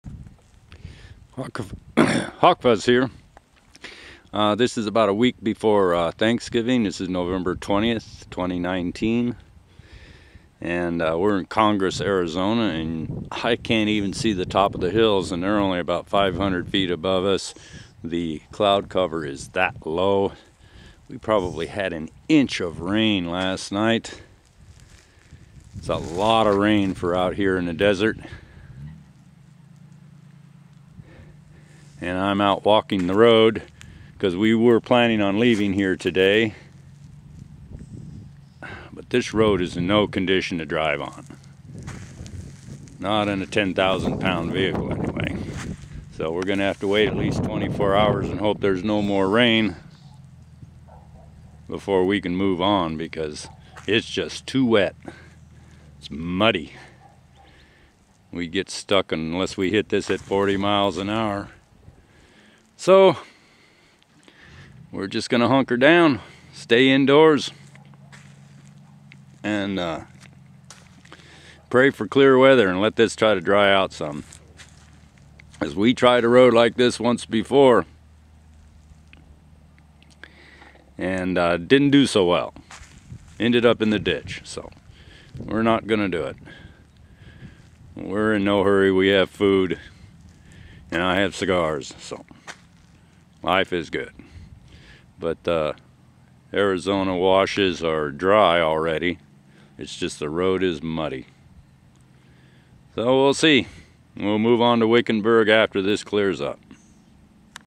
Congress_2019
Congress, Arizona on BLM land. Small, but nice with shopping nearby